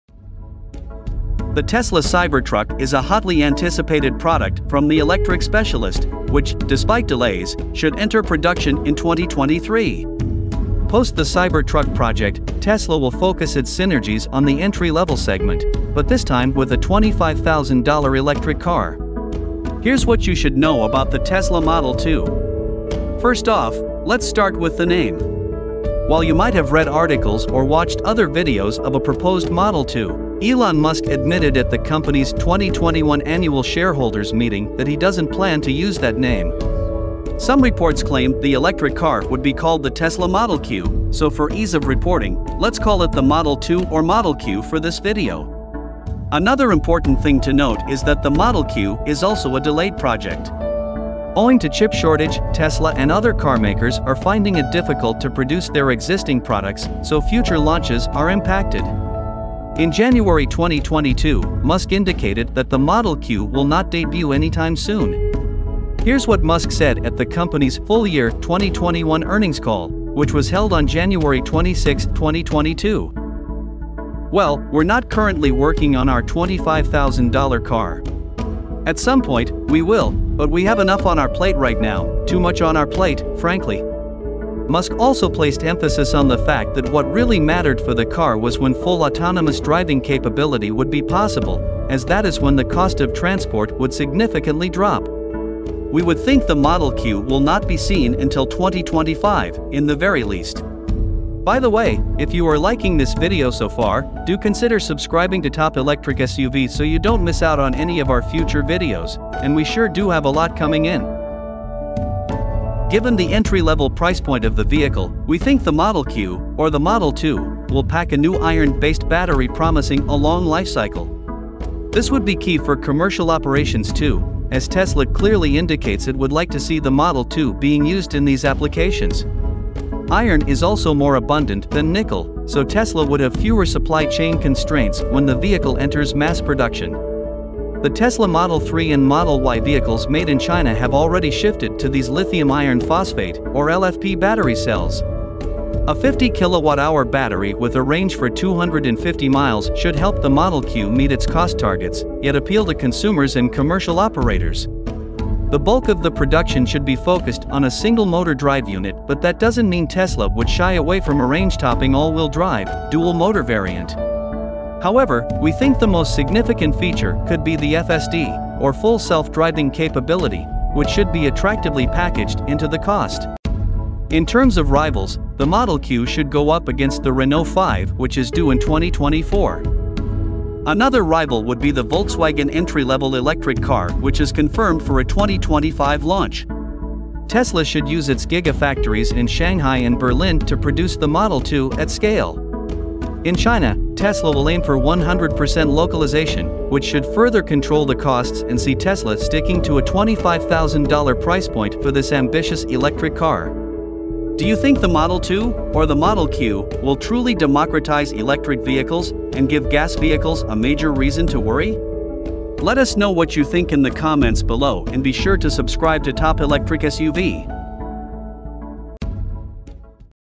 $25,000 Tesla Model 2/Model Q: Can we expect it in 2025?
By 2025, Tesla will be seen pushing the envelope at the low-end of its lineup by introducing an electric car for $25,000. Currently known as the Tesla Model 2 or the Tesla Model Q, the entry-level offering could truly democratize electric cars. It should be produced at Giga factories in Shanghai and Berlin and lock horns with the upcoming entry-level VW electric car, the Renault 5 EV and GM's future mass-market EV for America. Its 50 kWh battery should offer 250 miles driving range. A highlight of the package should be the Full Autonomous Driving Capability.

Thumbnail & renders: Instagram/sugardesign_1
Footage - Tesla